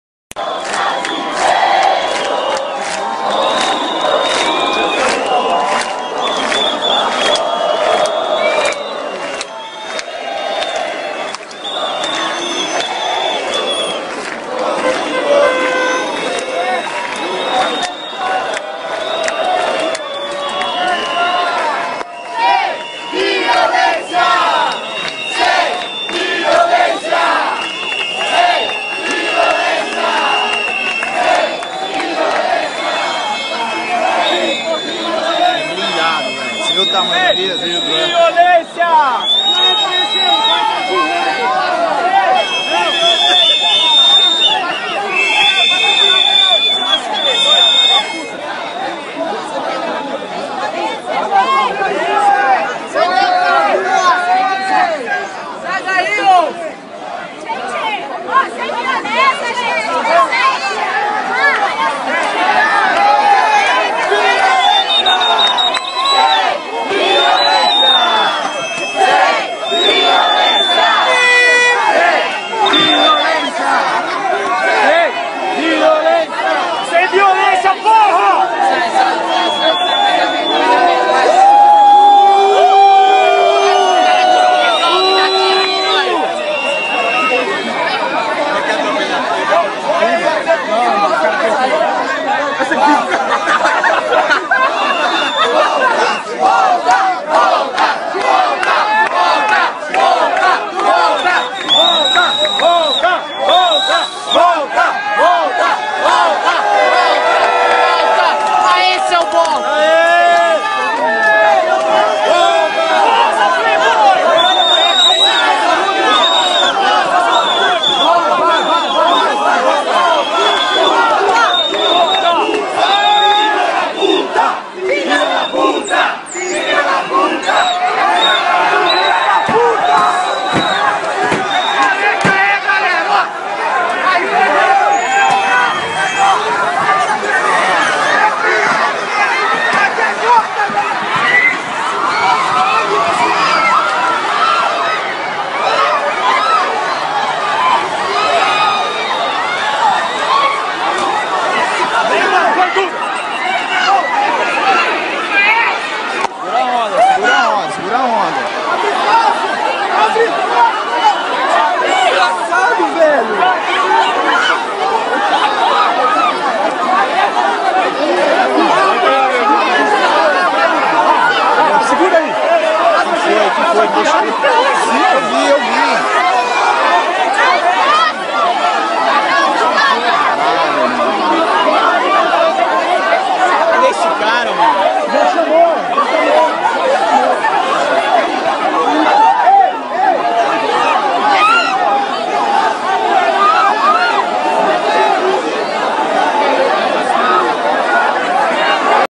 MOTORISTA Lutador de jiu-jítsu ATROPELA 13 pessoas E MATA Marcos de 18 anos
PRIMEIRA MORTE DOS PROTESTOS   A Polícia Civil de Ribeirão Preto, a 313 km, pediu nesta sexta-feira (21) a prisão do empresário Alexsandro Ichisato de Azevedo, de 37 anos, suspeito de ter avançado com seu carro e atropelado 13 pessoas na noite anterior, matando Marcos Delefrate, de 18 anos. O carro dele foi abandonado no condomínio de luxo em que reside, mas ele não foi encontrado no local e é procurado. Mais cedo, o advogado de Azevedo disse que ele se apresentaria fora de Ribeirão.

De acordo com o delegado Carlos Henrique Araújo, a Range Rover preta tinha marcas de sangue e foi mandada para Araraquara (SP) porque o pátio em Ribeirão está lotado. Segundo ele, mesmo que o empresário se apresente em outra cidade, todo o inquérito vai se desenrolar em Ribeirão e a polícia da cidade terá de ouvi-lo e ficará responsável pelo caso.

O empresário atua no ramo de revenda de imóveis e também teria uma academia de artes marciais onde seria professor. Lutador de jiu-jítsu, já responde a mais de dez processos na Justiça, alguns deles relacionados a agressões. Em vídeos feitos pelos manifestantes, ele acelera o carro duas vezes para passar pelo bloqueio na avenida João Fiúza, em vez de dar ré e retornar. Na terceira vez, acelera com tudo e vai atropelando quem está pela frente.

Motorista que matou manifestante em Ribeirão Preto vai se apresentar à polícia

MPL anuncia que não convocará novos protestos em São Paulo

No local onde as pessoas foram atropeladas e o jovem morreu, foram colocados flores, cartazes, velas, mensagens e uma bandeira do Brasil. Até por volta das 12h30, duas jovens seguiam internadas e uma delas, em estado mais grave, precisará passar por cirurgia.

Luto

A Prefeitura de Ribeirão Preto decretou luto de três dias pela morte de Marcos Delefrate. A prefeita Darcy Vera (PSD) afirmou que "a tragédia trouxe indignação e tristeza para todos".

— Uma manifestação pacífica e organizada, que tinha tudo para marcar a história de nossa cidade e servir de exemplo para a democracia do País, deixou uma grande cicatriz em todos nós.